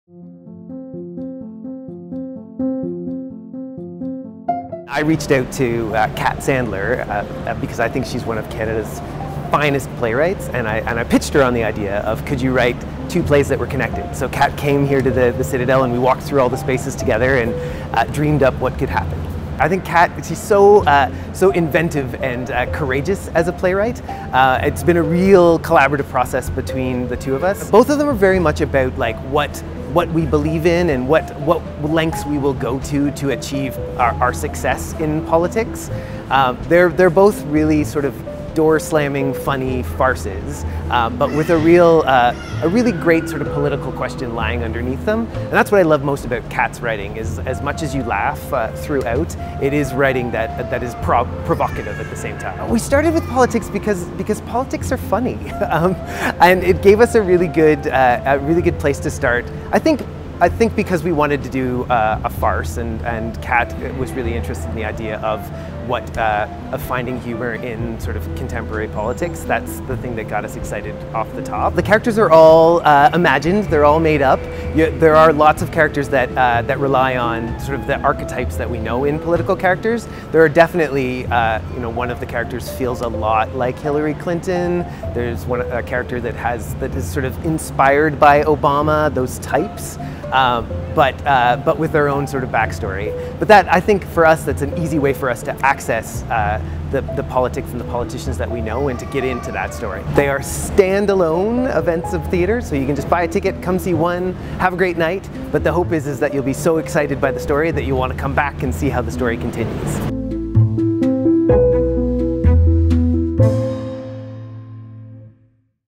"Politics are Funny!" - Daryl Cloran on The Candidate/The Party
Today, one brave cast started rehearsals on two bold and hilarious comedies running at the exact same time! The shows tackle the farcical reality of our political climate, poking fun and asking who's really in charge in our world. This is a theatrical event unlike anything we've seen in our building, join in on the fun! Really, what could go wrong?

Kat Sandler's brand-new scripts, The Candidate and The Party, take over The Club and the Maclab Theatre March 30-April 21.